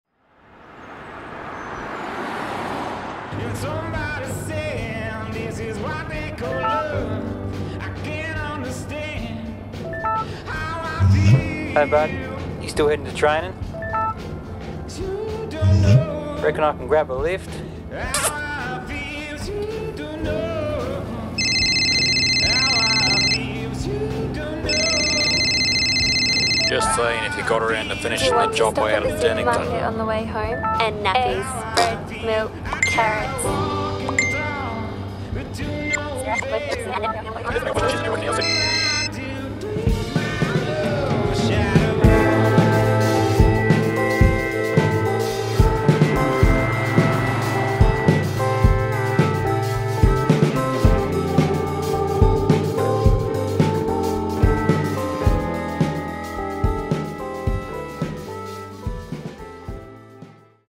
No Distraction 3280
Keeping your eyes on the road is essential for safe driving. But when you look at your phone, you’re oblivious to what’s around you. In fact at 50km per hour, even a 4 second glance at your phone means you travel almost 50 metres blind. What will you miss?